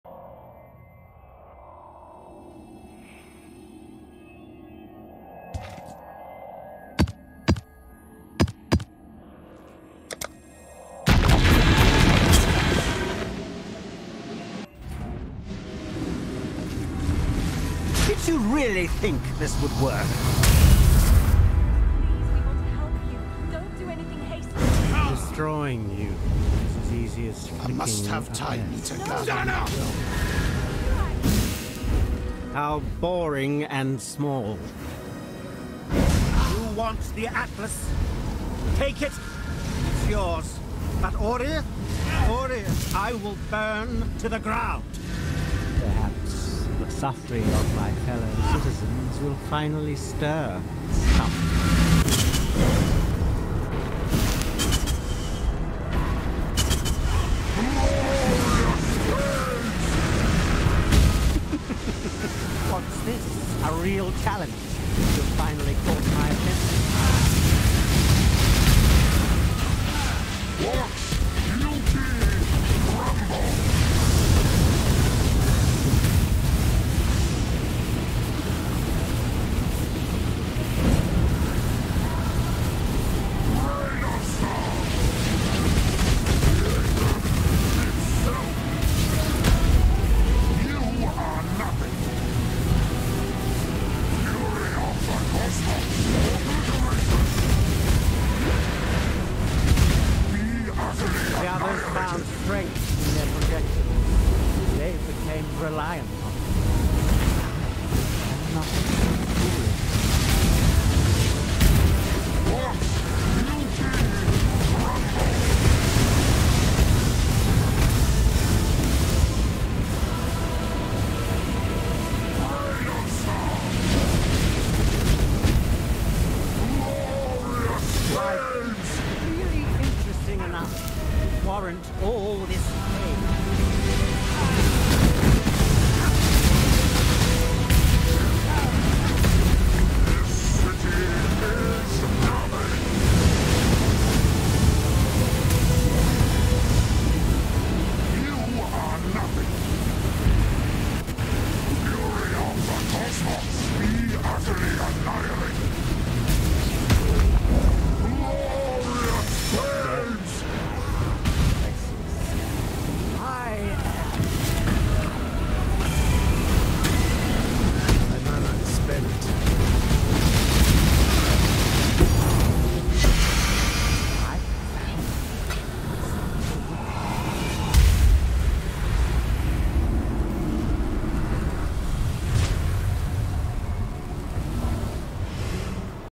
3.21 - Blazing Salvo Totems Hierophant - Uber Sirus Deathless
My first uber Sirus of the league, now 7/7 ubers and 40/40 challenges.